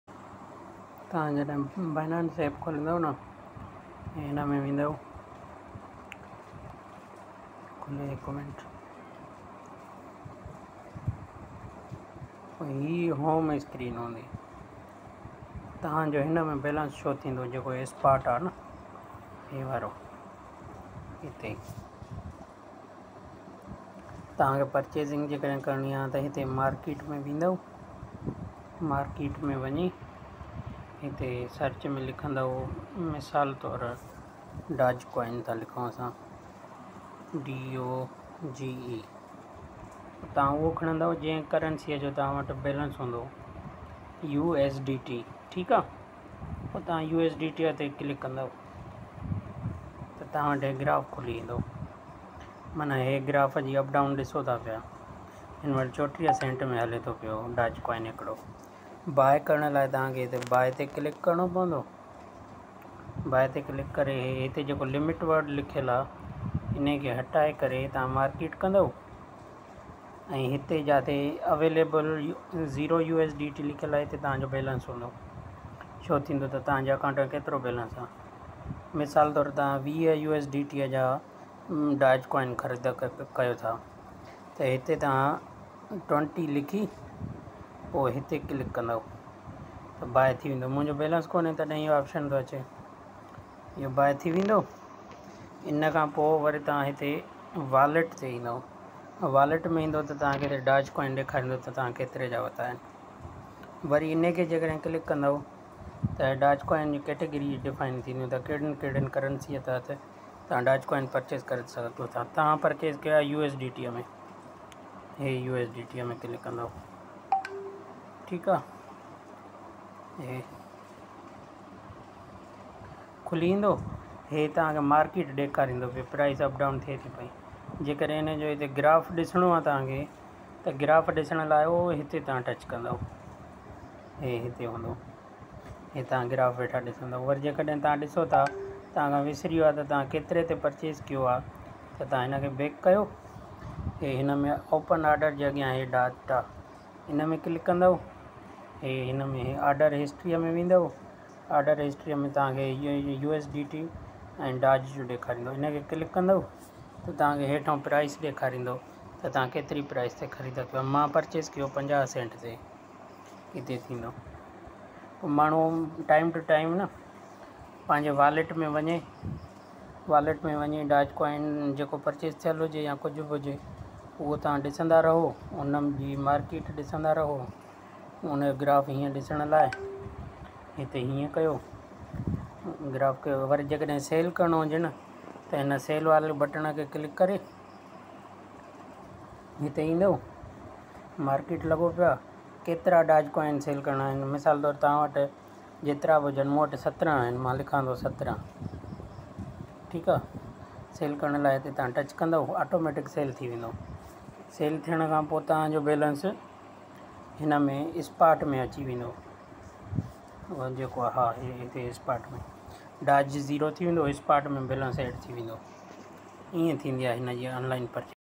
Binance Application|purchase| crypto currency| sindhi lesson|online earning|trading| guideline
binance application pe crypto currency purchase karny ka tareeka sindhi zuban mein. purchase currency ka graph aur balance dekhny ka tareeka .
crypto currency sell karny ka tareeka.